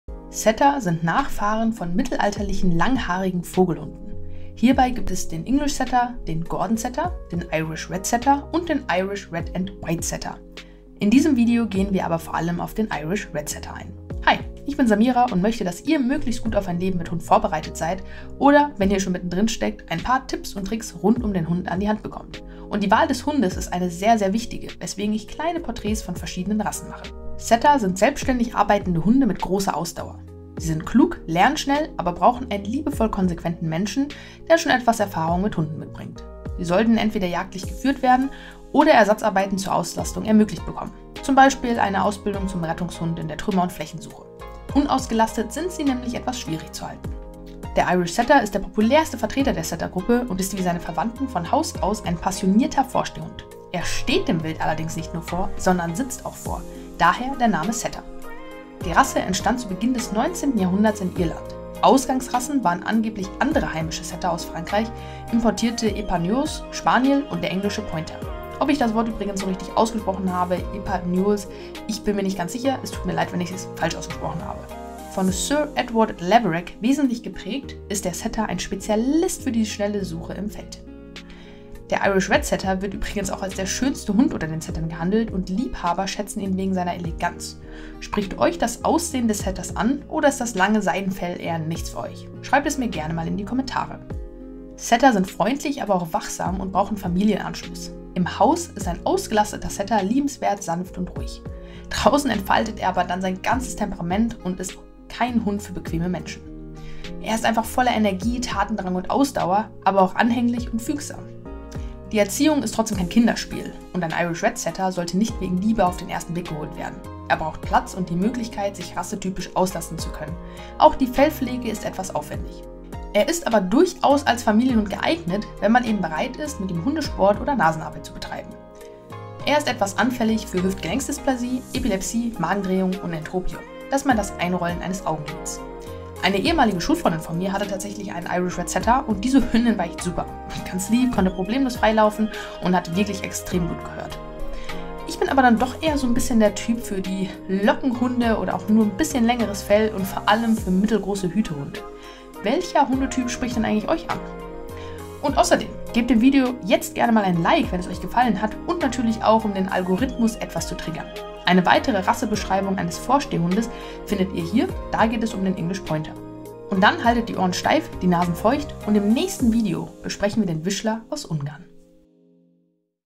IRISH Setter - Geschichte, Charakter & Haltung [Rassecheck]
Der Irish Setter bringt eine elegante Erscheinung mit sich, aber auch viel Temperament und Ausdauer. Erfahrt mehr über den Vorsteher in diesem Video.

Hi, ich bin Samira und finde eine verantwortungsvolle Auswahl des Hunde sehr wichtig. Hierbei geht es nicht um ein Statussymbol oder einem Modetrend, dem man hinterher läuft. Hier geht es um ein Lebewesen.

Und der Irish Setter ist kein Hund für bequeme Menschen. Er ist ein Jagdhund und klassischer Vorsteher, bzw. Vorsitzer. Mit Training und liebevoller Konsequenz ist er aber gut zu erziehen und auch gut als Familienhun geeignet. Wenn ihr wissen möchtet, ob der Irish Setter zu euch passt, schaut euch gerne das Video an.

Fakten zum Irish Setter:
Lebenserwartung: 12 bis 15 Jahre
Größe: Weiblich: 55–62 cm, Männlich: 58–67 cm
Gewicht: Weiblich: 24–29 kg, Männlich: 27–32 kg
Farben: sattes kastanienbraun

0:00 Einleitung
0:27 Wesen des Irish Red Setter
0:58 Geschichte des Irish Setter
1:34 Charakter  und Erziehung des Irish Setter
2:08 Gesundheit des Irish Setter
2:15 Meine Erfahrungen und Meinung zum Irish Red Setter

DINDXKP29ZOE340I